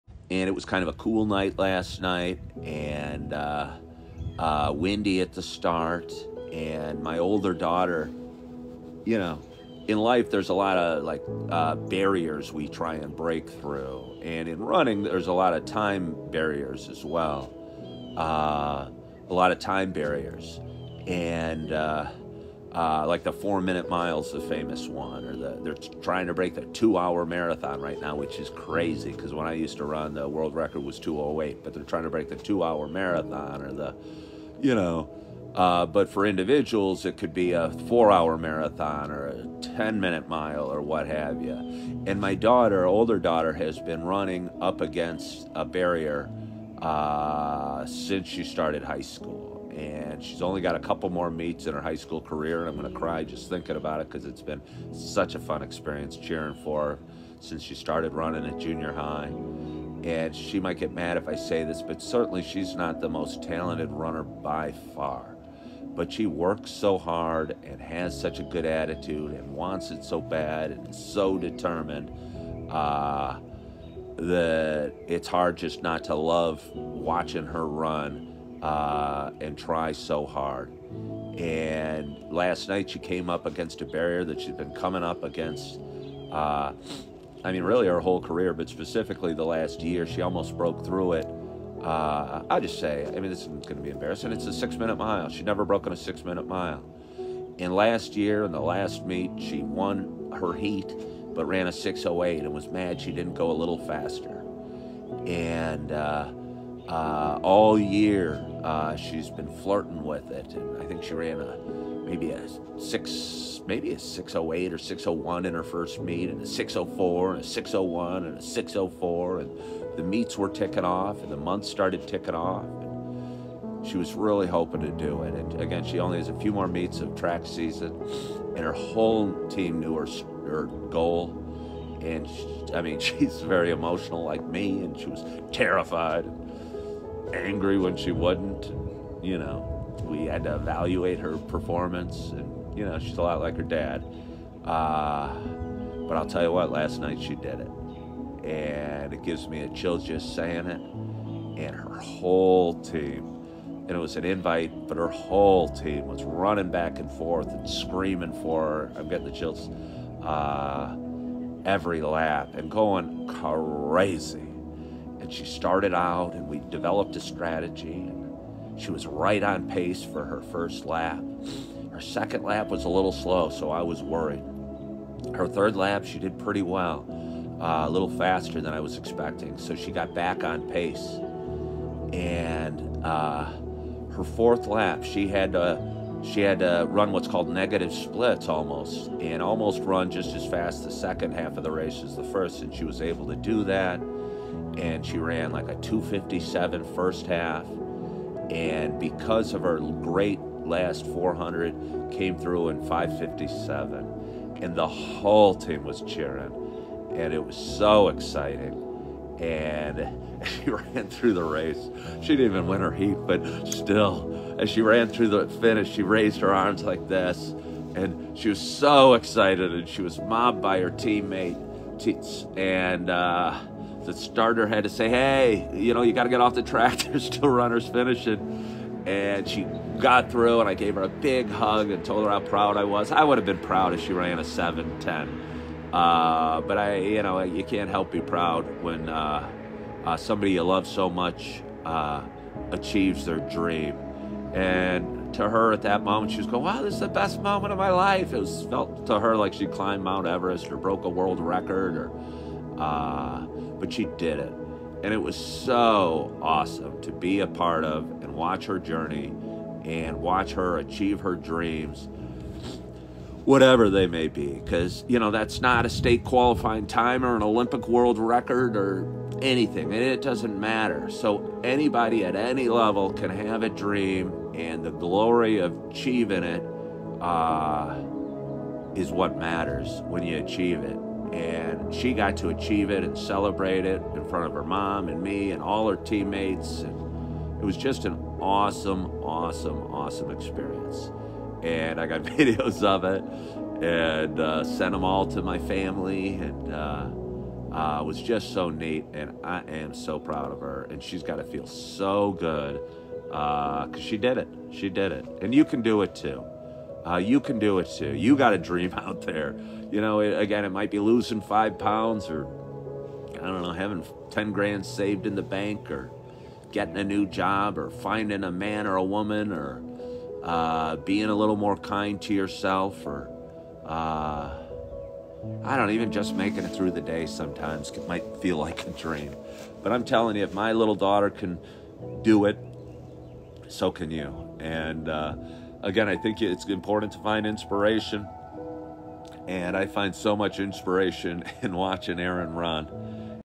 Achieving a dream: You can do it too…
Join me for a chat about my daughter Erin, who broke through a barrier and ran her her best mile race ever…
It is important to draw inspiration in life, and I have been inspired watching Erin compete in track and achieve her dreams…